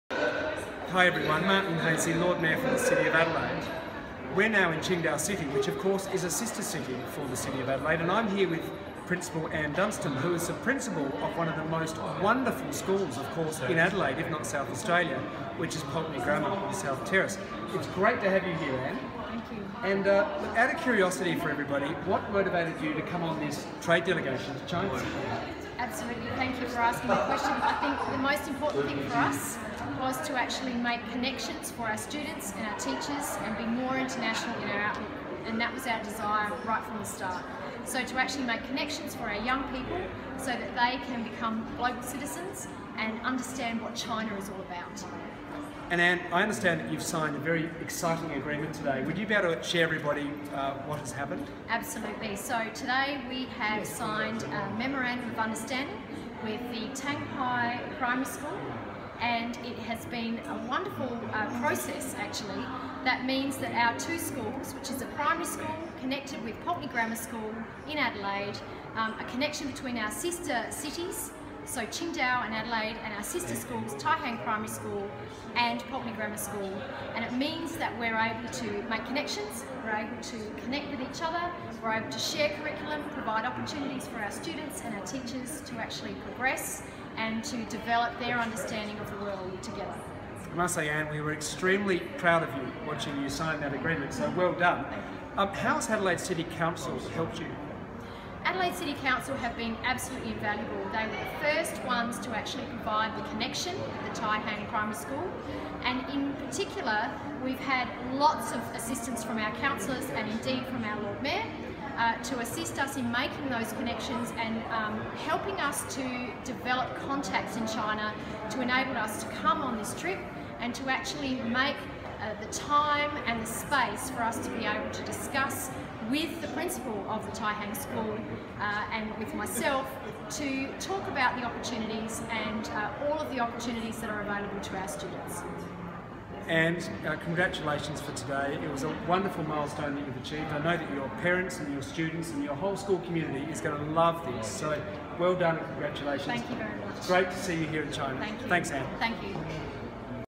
Lord Mayor Martin Haese interviews Pulteney Grammar Principal Anne Dunstan
City based students will become true global citizens with Pulteney Grammar School signing an MOU with Huangdao District Taihang Road Primary School in Qingdao as part of the South Australian Trade Mission to Shandong.
The MOU creates opportunities for student and teachers to undertake exchanges and learn about each other's culture.